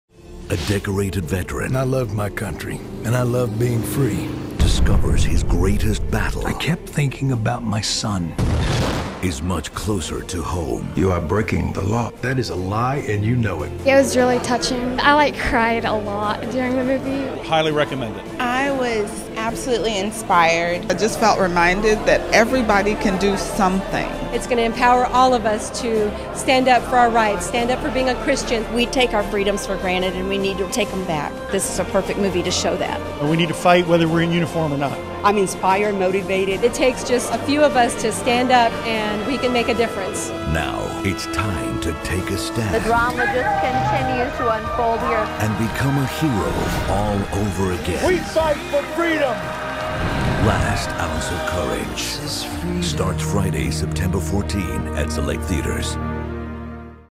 What People are Saying about Last Ounce of Courage
Last Ounce of Courage is an intergenerational story about family, free expression, and taking a stand for cherished beliefs. A family is struck by tragedy when a young man is lost in combat overseas, leaving behind a wife and young son. Years later, the fallen soldier's father, Bob Revere, serves as mayor of a small town where powerful Washington interests seek to stamp out religious expression. The soldier's son, now a young man, inspires his grandfather to take a stand for the ideals and traditional values that our heroes in uniform sacrifice everything to protect. In honor of his son, Revere embarks on a personal mission to reignite the latent patriotism and faith of his fellow citizens and reassert the cherished rights guaranteed to all Americans.

Releases nationwide September 14, 2012.